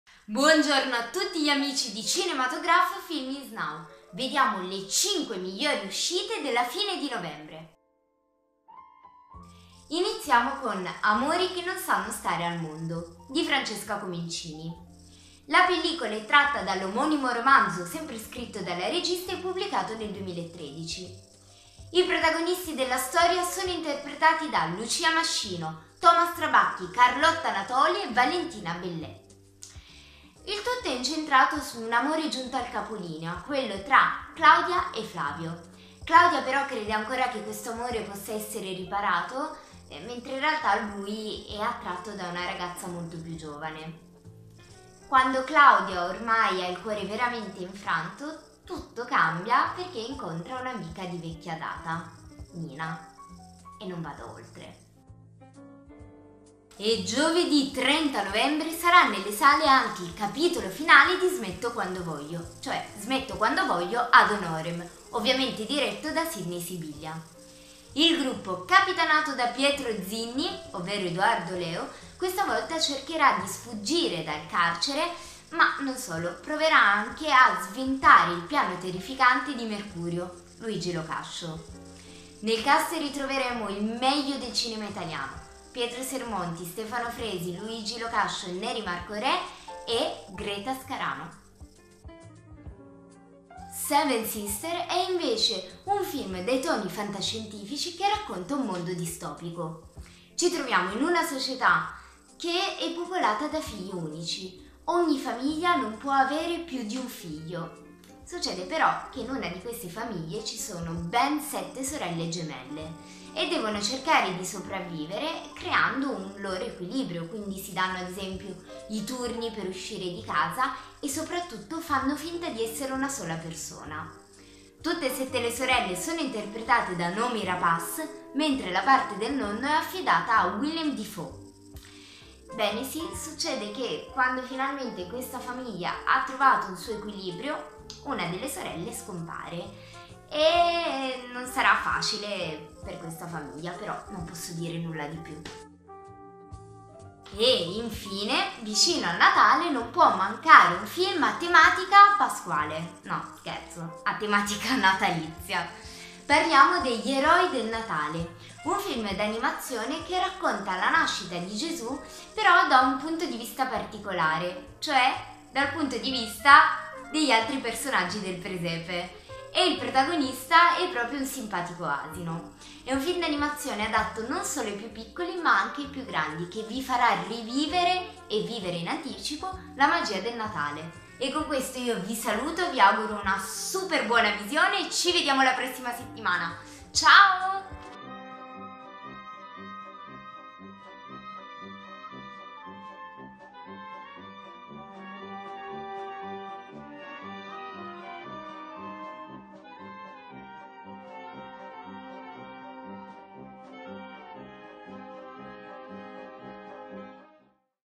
SEVEN SISTERS e SMETTO QUANDO VOGLIO - AD HONOREM | Cosa vedere al cinema questa settimana
Il Manuale del Cinefilo #26 - Amori che non sanno stare al mondo, Seven Sisters, Smetto quando voglio - Ad honorem, Gli eroi del Natale . Kiara vi consiglia i film da vedere (e non perdere) al cinema da Giovedì 30 Novembre 2017.